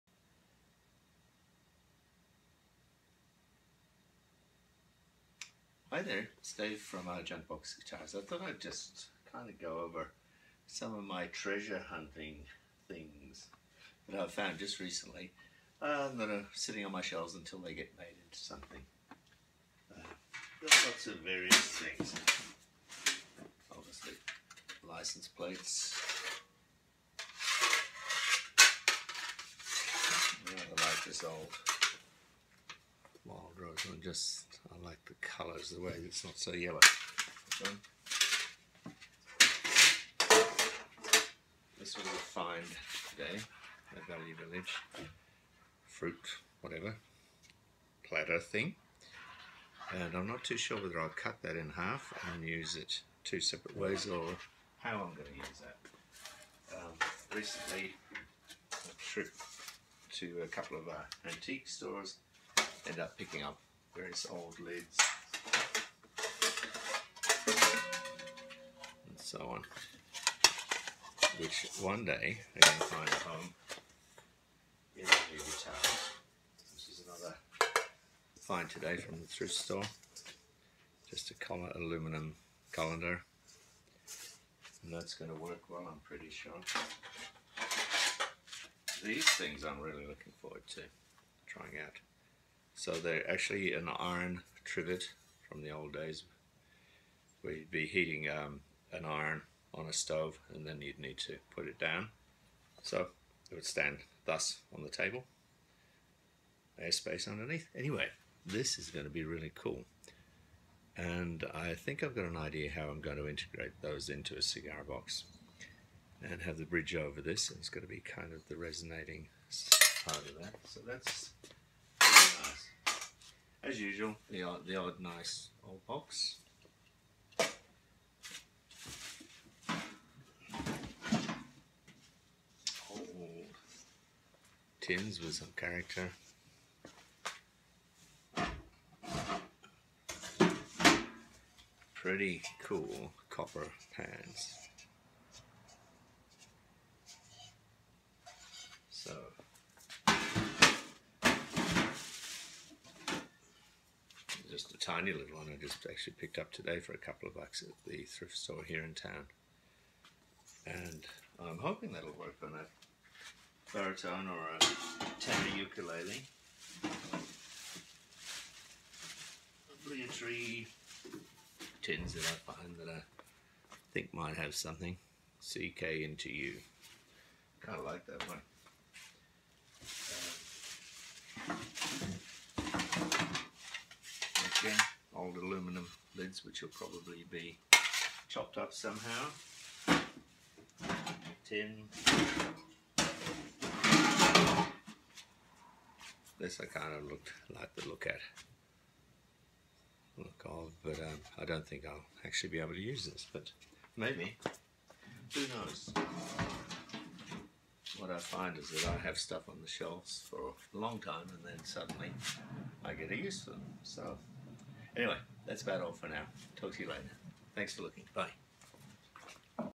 Finding materials for a cigar box guitar
Some of the treasures that I've found lately...just waiting to become a Junkbox  Guitar